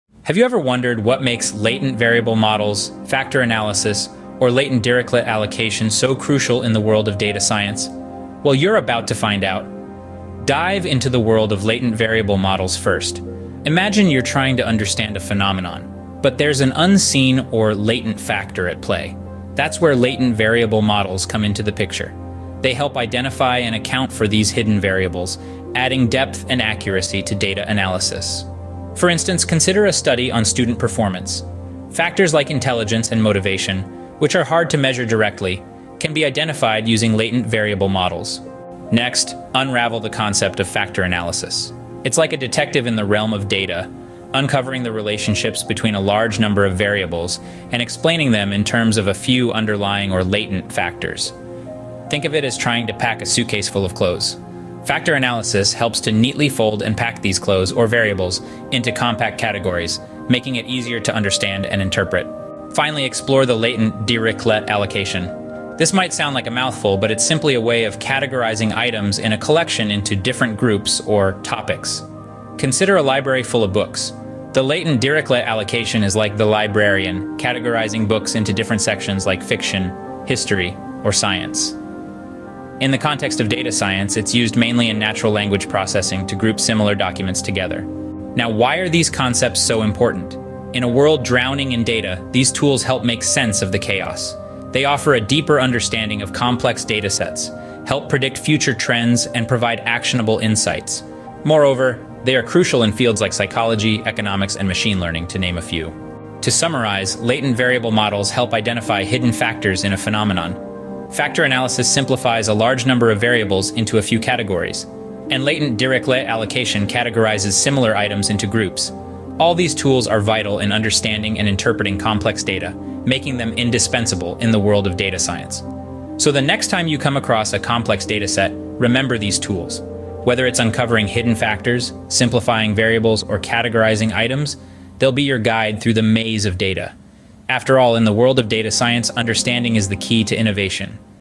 Latent Variable Models
Dive into the fascinating world of latent variable models in this comprehensive tutorial. We'll start by exploring the concept of latent variables and their importance in modeling hidden structures within data. Then, we'll delve into Factor Analysis, a technique used to identify underlying factors that explain observed correlations in data. Next, we'll cover Latent Dirichlet Allocation (LDA), a popular topic modeling algorithm that uncovers latent topics in text corpora. By the end of this video, you'll have a solid understanding of how to apply these powerful models to uncover hidden patterns and structures in your data analysis and machine learning projects.